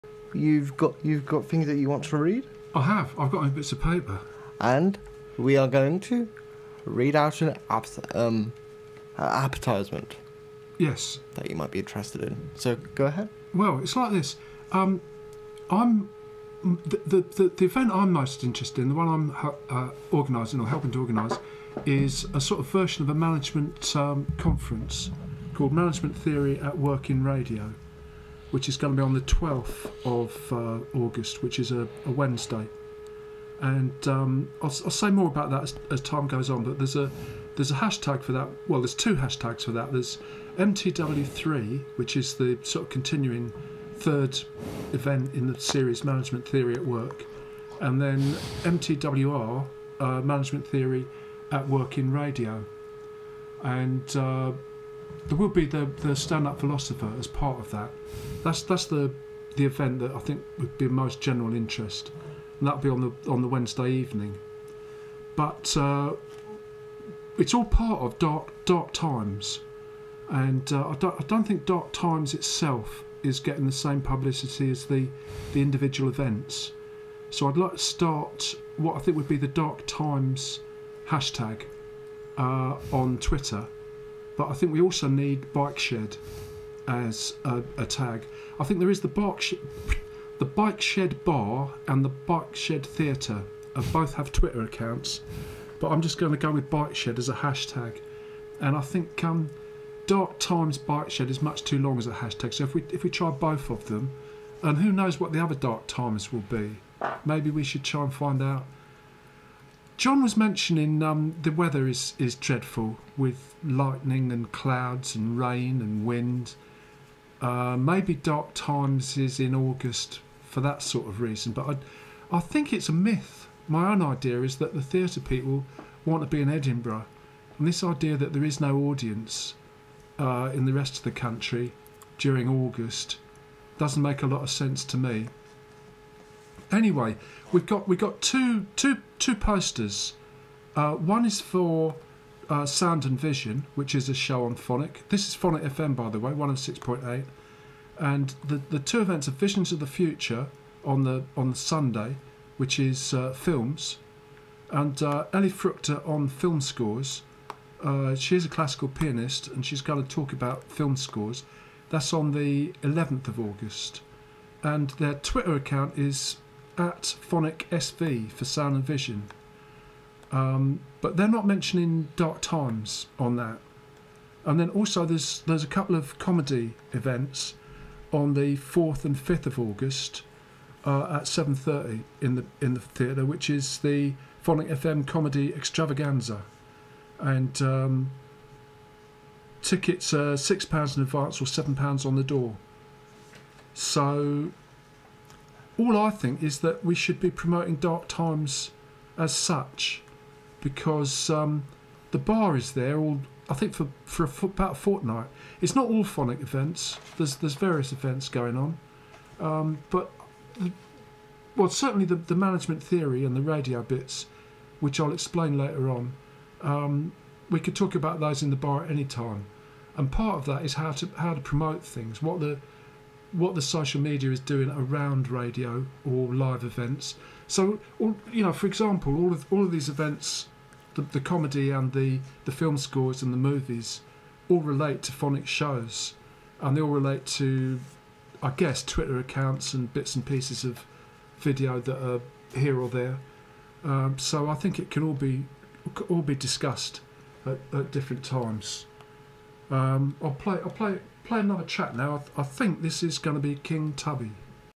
darktimes mention on @wenotno
Clip from @wenotno proposing more profile for dartimes as a tag and an occasion, to include various events as some sort of overall concept. To include also mtw3 and mtwr , a sort of management theory day. Central case study around how a YouTube playlist and social media can work as promotion and find a video production budget .